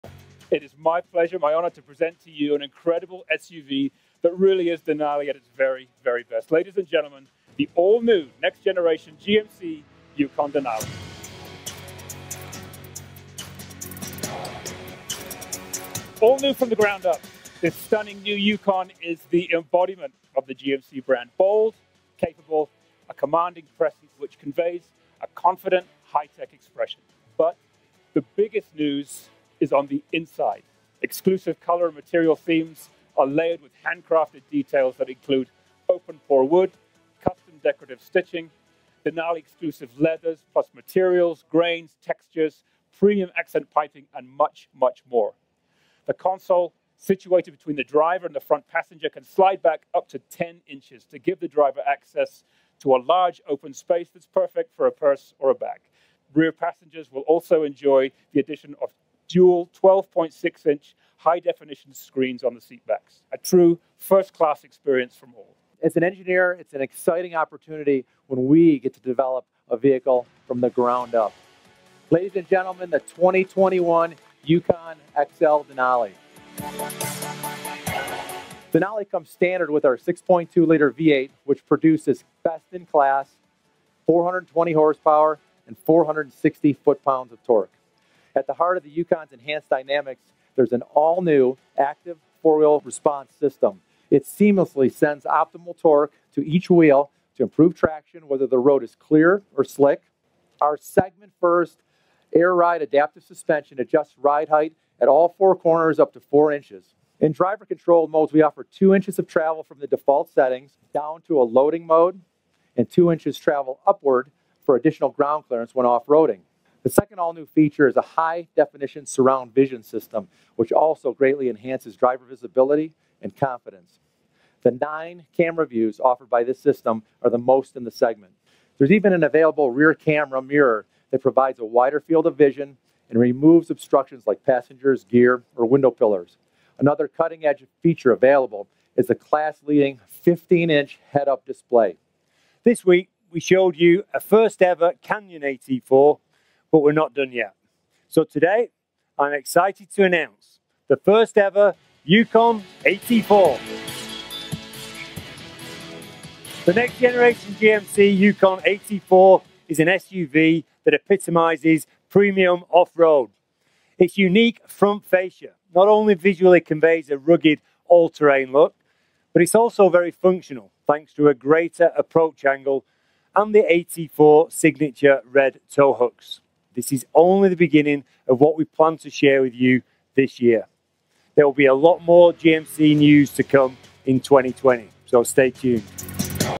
2021 GMC Yukon Denali And Yukon AT4 Full Inside | Out First Look
2021 GMC Yukon Denali And Yukon AT4 Debut

A fresh design, a new AT4 off-road trim, and at last a Denali-appropriate interior cap off the changes.